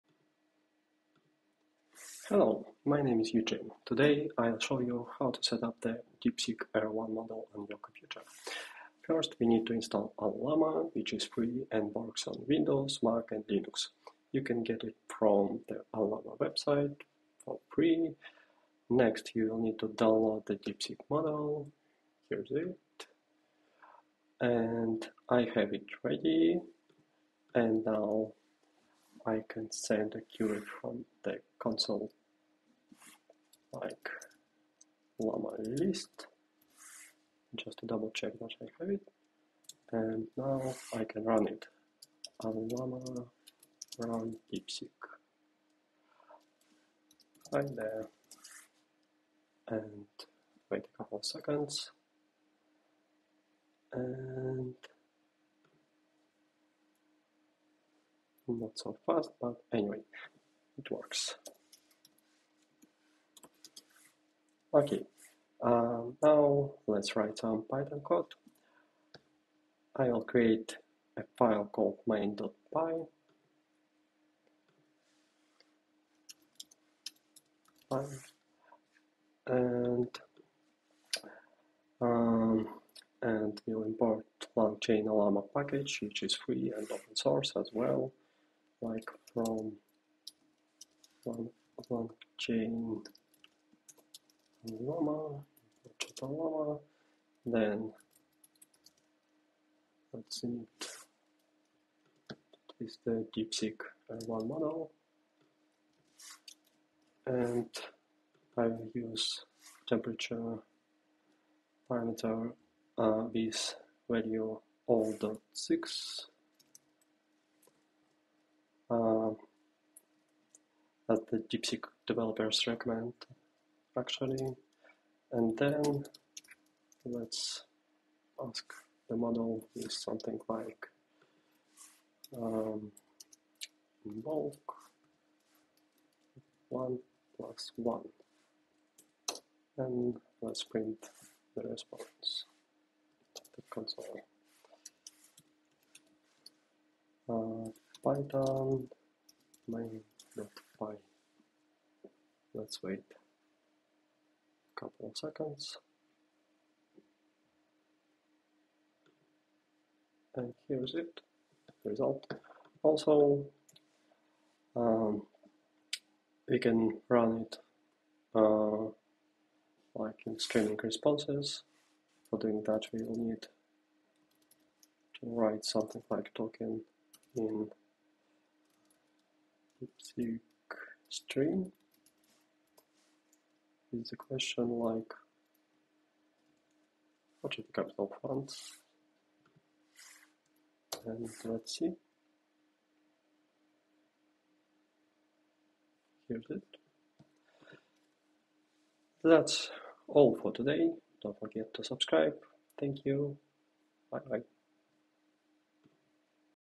Quick Setup Guide for DeepSeek R1: Run AI Locally on Any OS!
I walk you through a simple setup to run the powerful DeepSeek R1 model on your Windows, Mac, or Linux system. In this quick tutorial, I'll share the necessary code, demonstrate how to initiate the model with optimal settings, and show you both single query and streaming responses. Perfect for beginners looking to dive into AI!